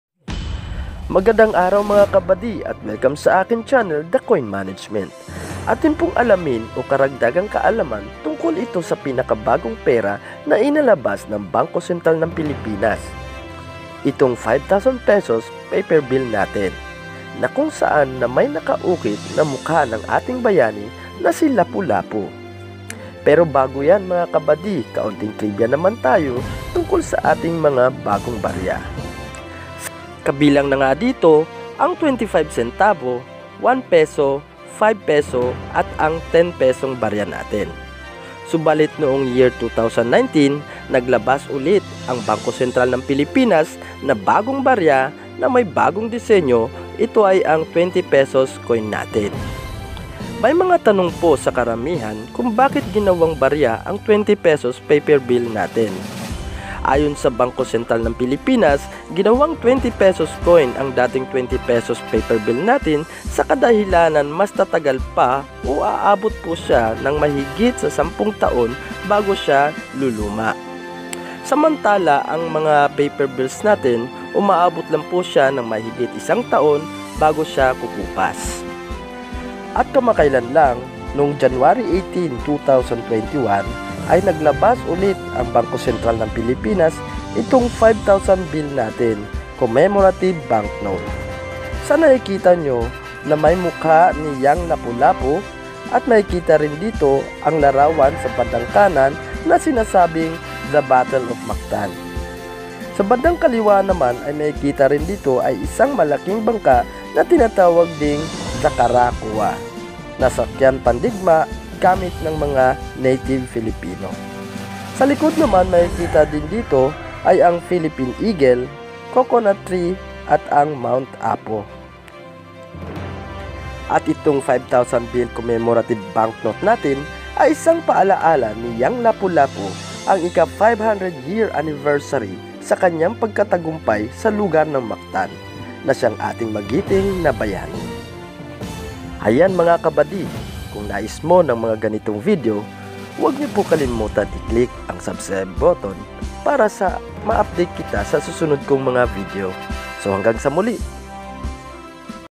5,000 pesos commemorative banknote|500th anniversary
Ang video ay naglalarawan tunkol sa pagbigay kaalaman o impormasyon sa bawat mga lumang barya na dapat natin malaman,ang mga history sa bawat pera.